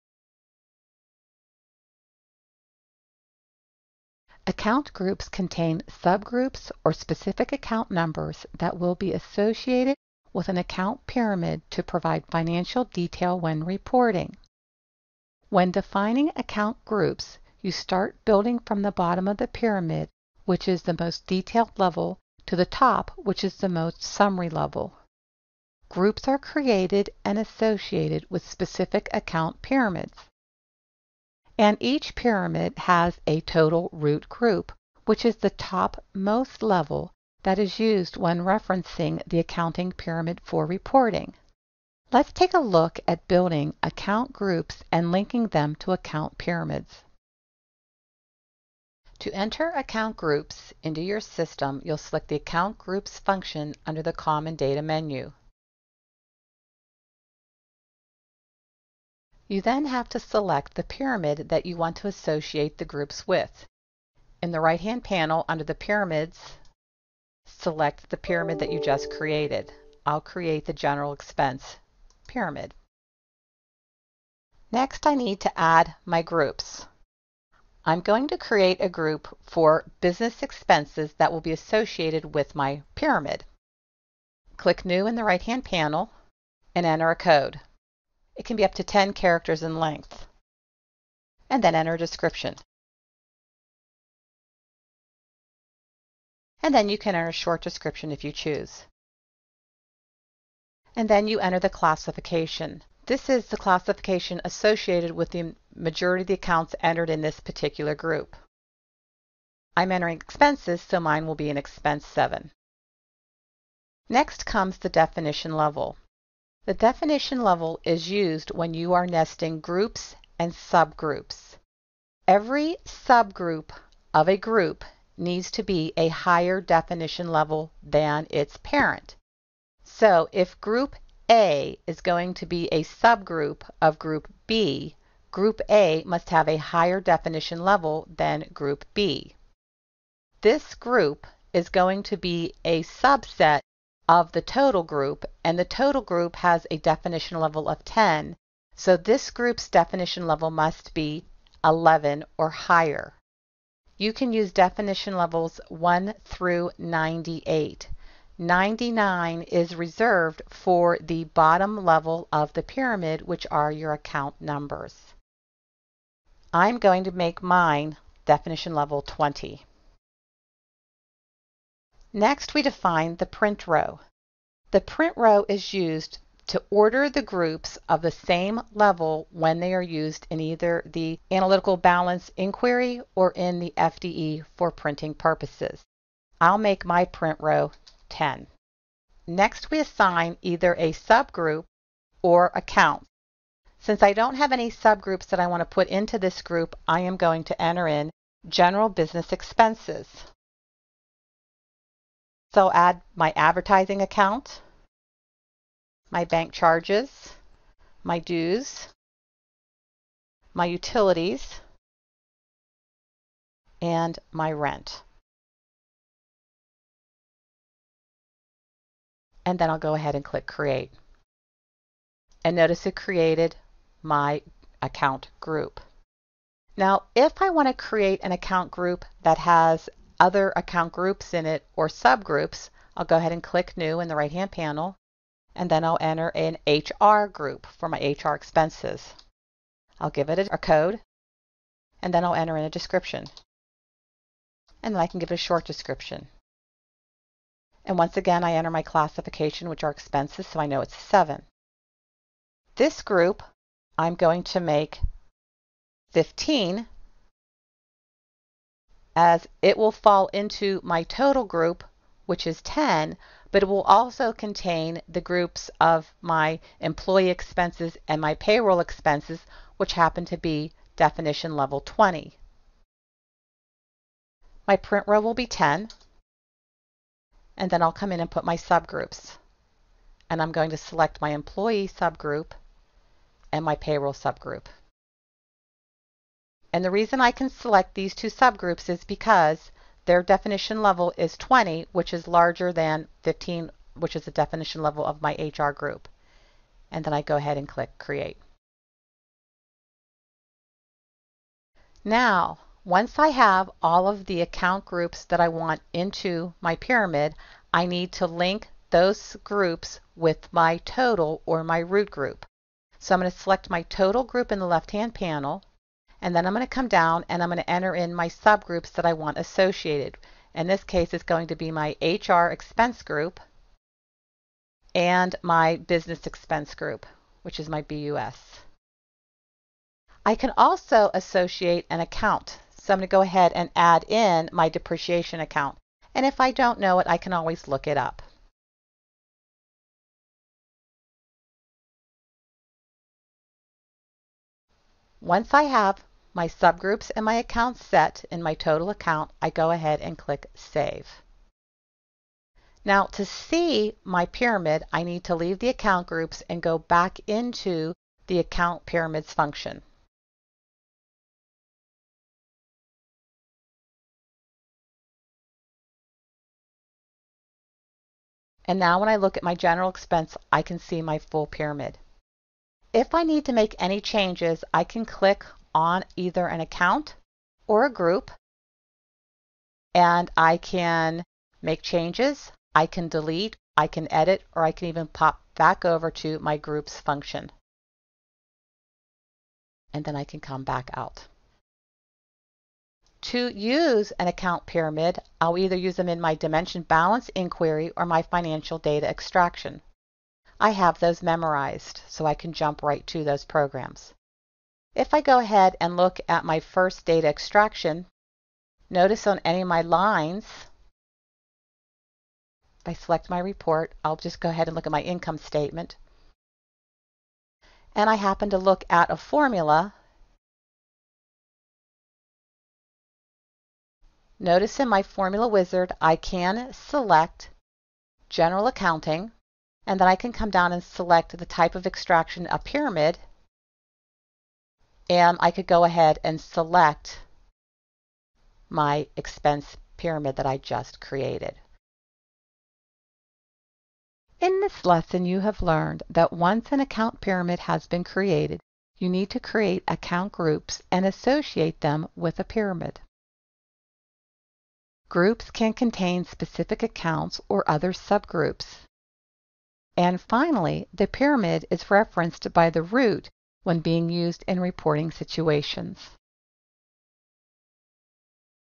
Sage X3 - How to Create Account Groups
In this Anytime Learning topic, you will learn that once an account pyramid has been created you need to create Account groups and associate them with a pyramid code. Groups can contain specific accounts or other sub-groups. And finally, the pyramid is referenced by the root when being used in reporting situations.